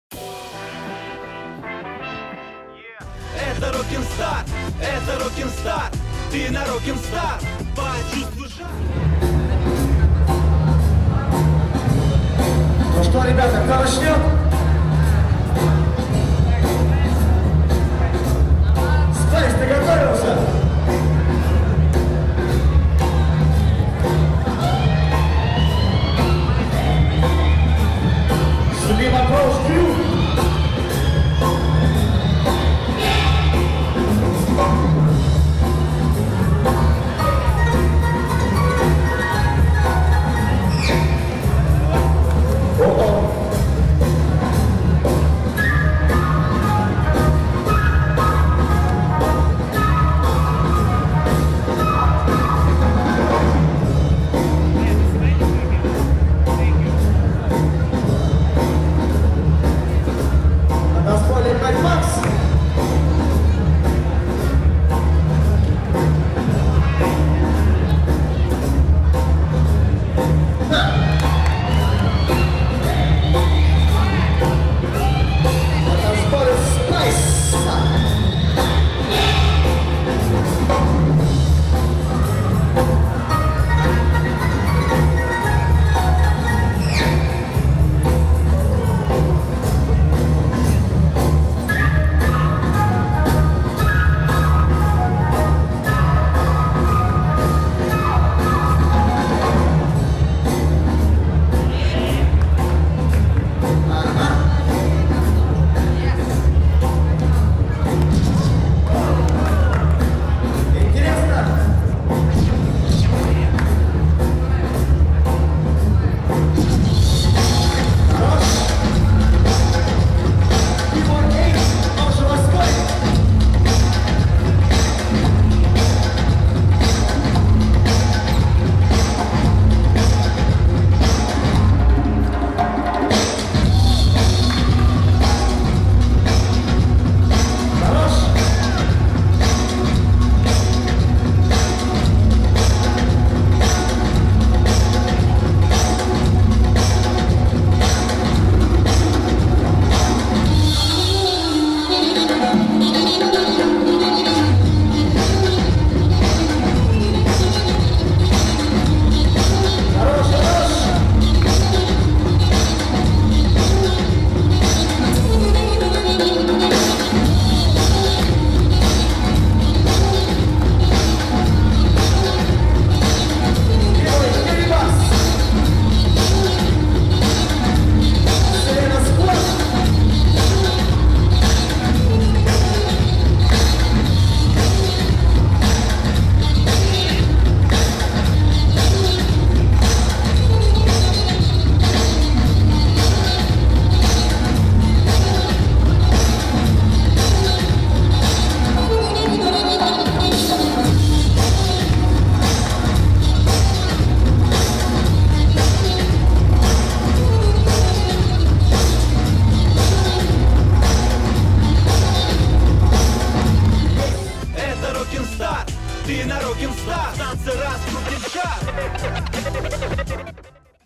ROCKIN' STAR 2017 | BREAKING | SELECT 2x2 PRO | Slim Approach Crew vs Selena Squad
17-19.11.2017
Международный танцевальный хип-хоп фестиваль "Rockin' Star" 2017
Творческое объединение "United Zoo"
Брейк-данс команда "Мастера Ситуаций"
Магазин уличной одежды Keepeshop
Keepershop.ru